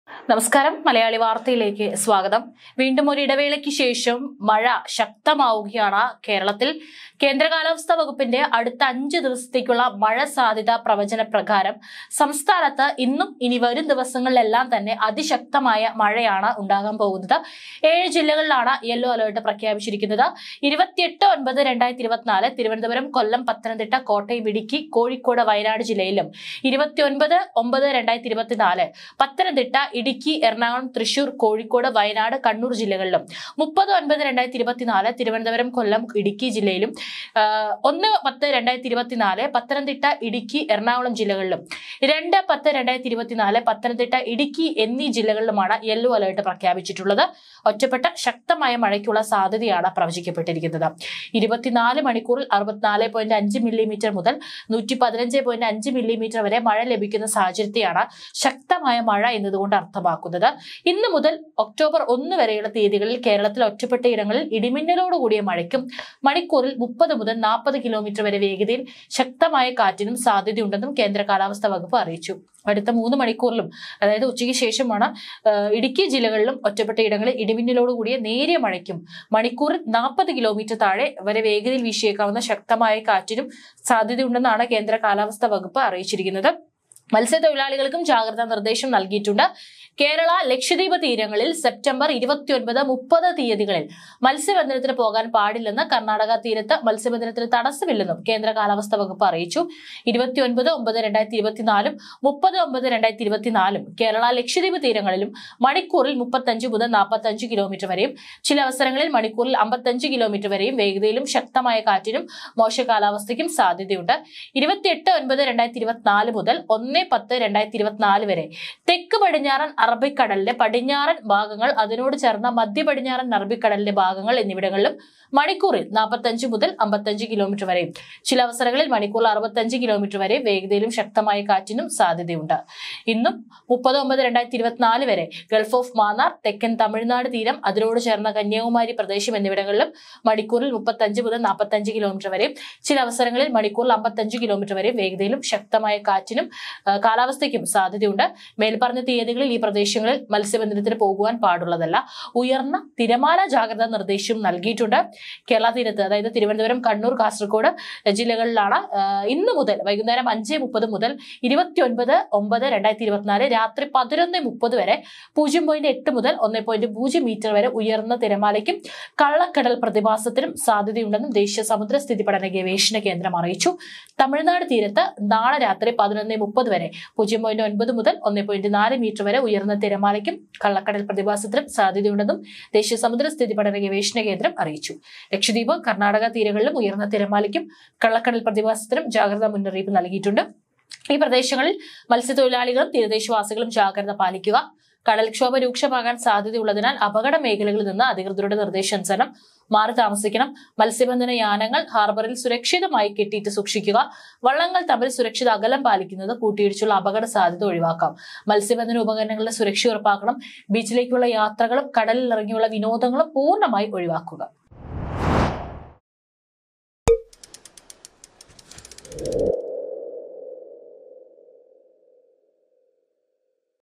RAIN ALERT ഒരാഴ്ച ശക്തമായ മഴ;
​ @malayalivarthakerala 2024

വരും മണിക്കൂറിൽ
ശക്തമായ മഴയും ഇടിമിന്നലും!
ഈ ജില്ലകൾ സൂക്ഷിക്കുക..
ജാഗ്രതയോടെ കേരളം...